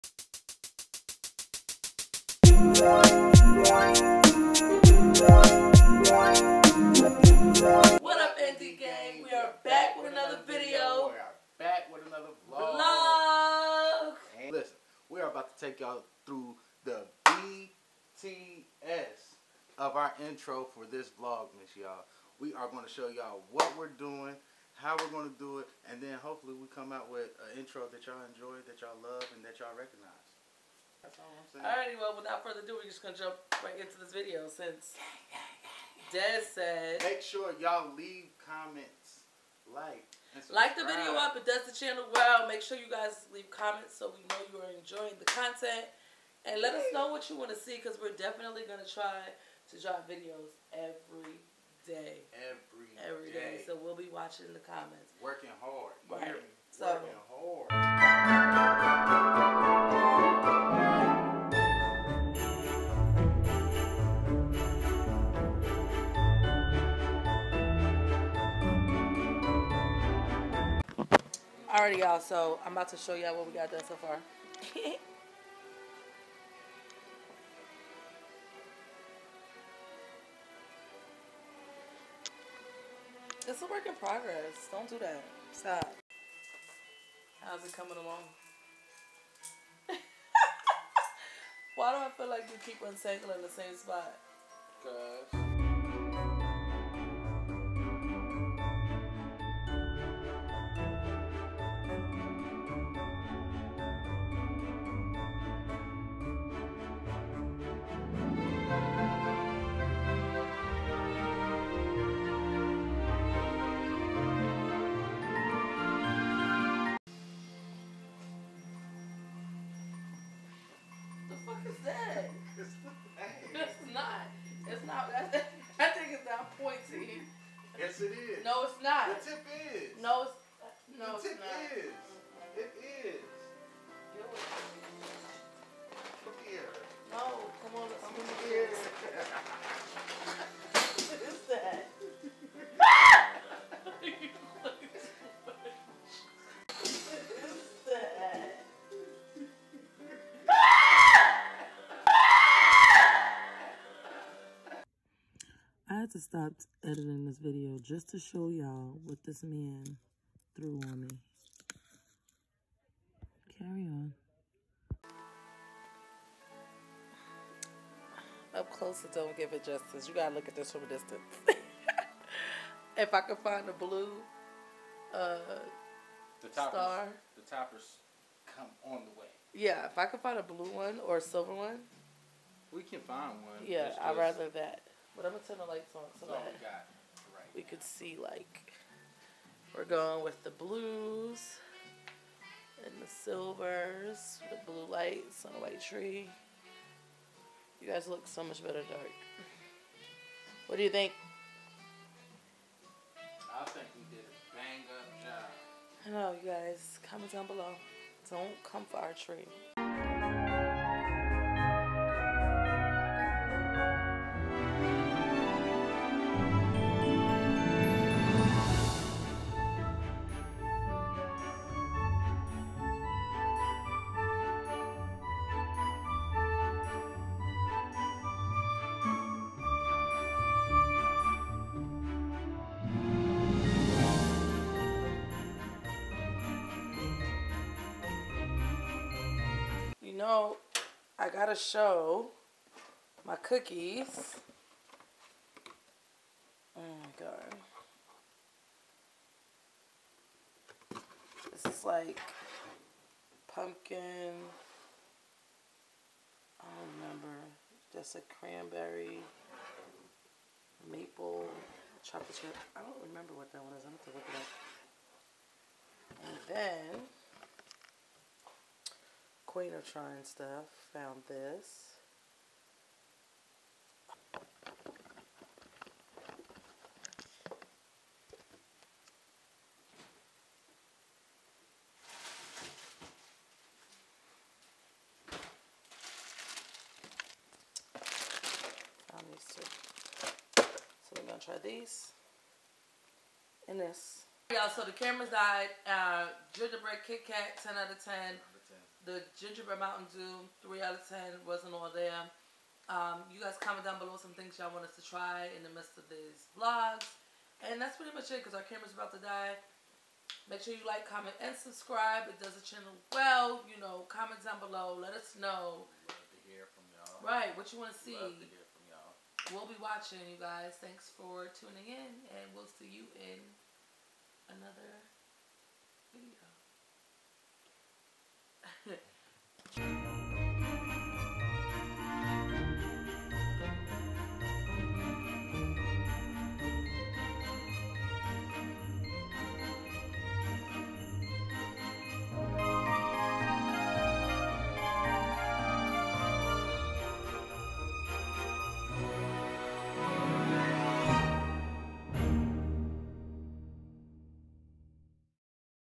Christmas Decoration| Putting up Our Tree| Vlogmas Day 1
Christmas decorations and putting up our tree. its vlogmas day 1 and we are excited for our first vlogmas.